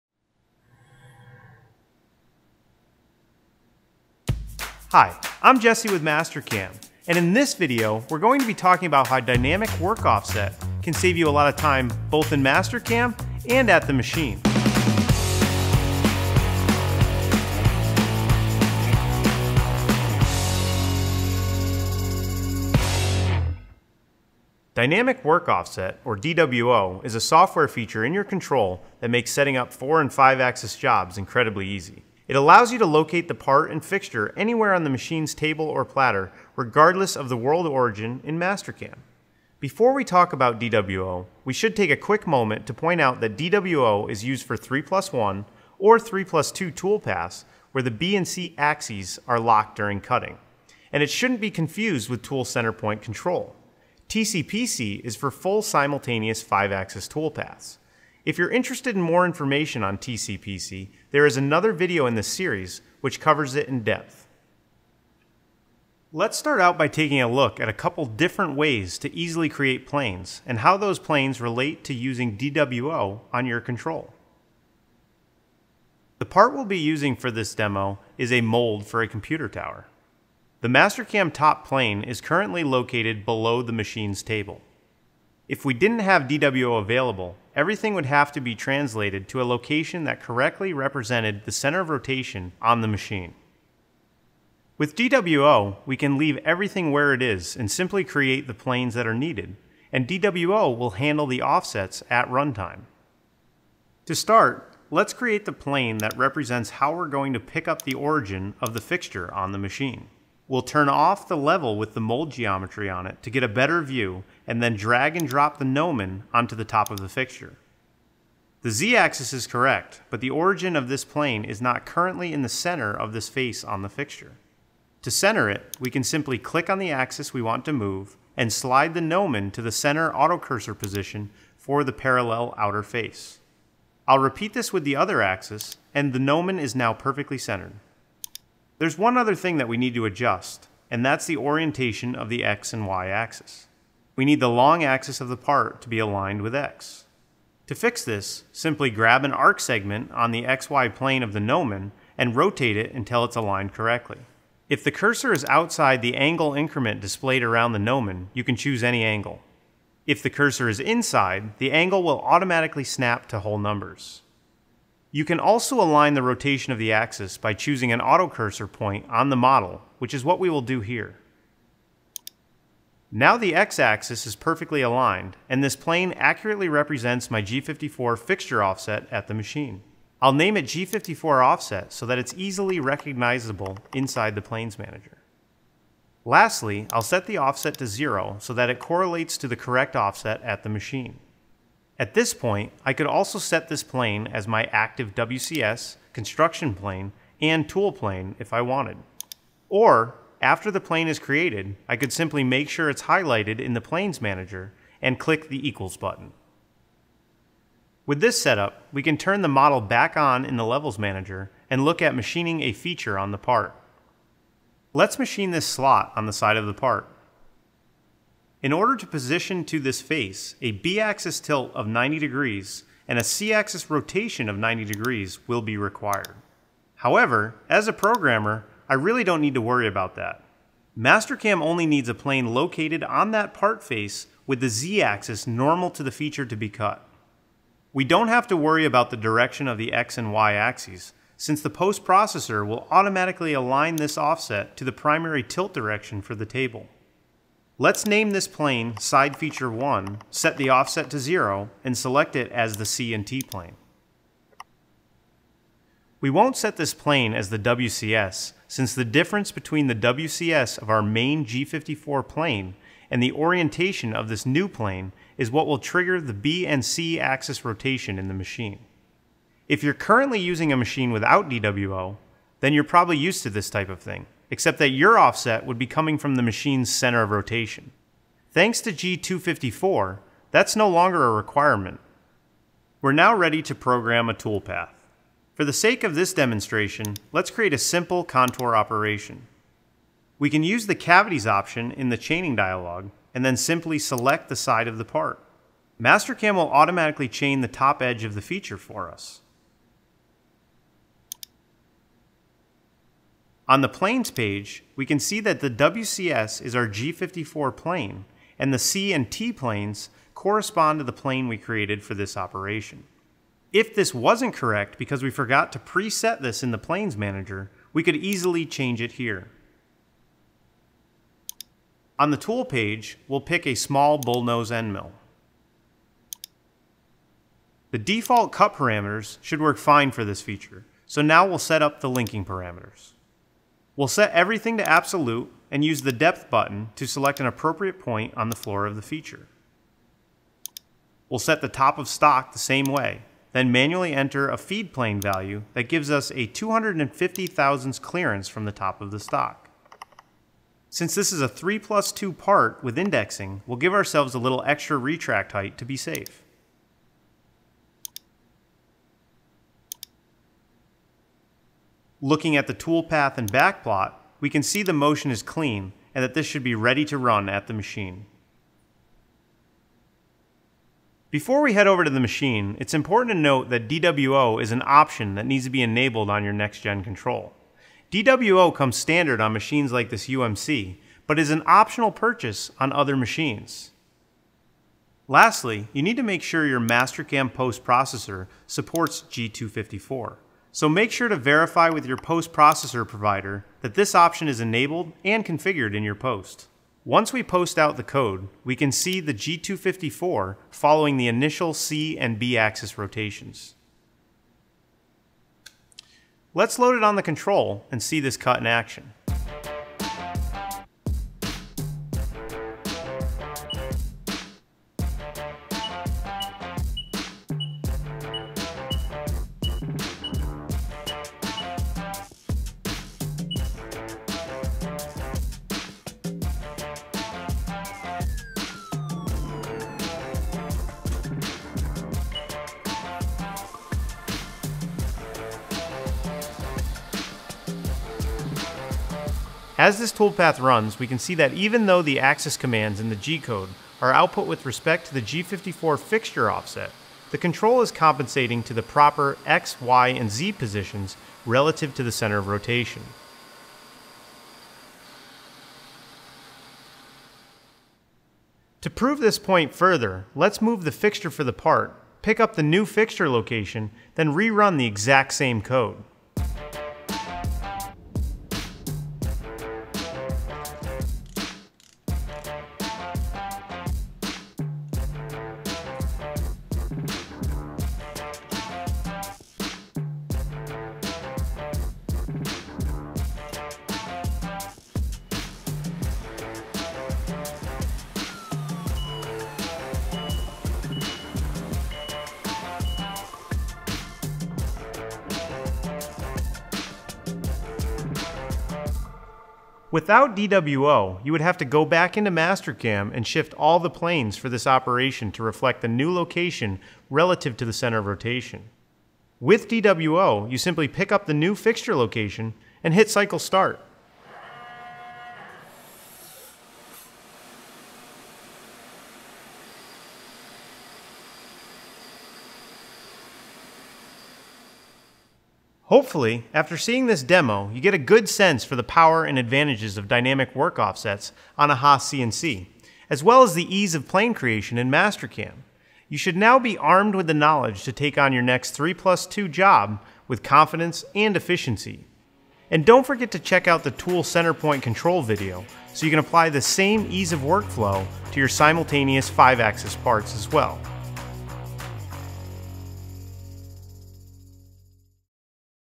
Dynamic Work Offsets - Make the transtition to multiaxis.
Transition your 3-axis programming skills to the world of multiaxis with Dynamic Work Offsets.
Download this part file to follow along in standalone Mastercam, sign into my.mastercam.com and follow this link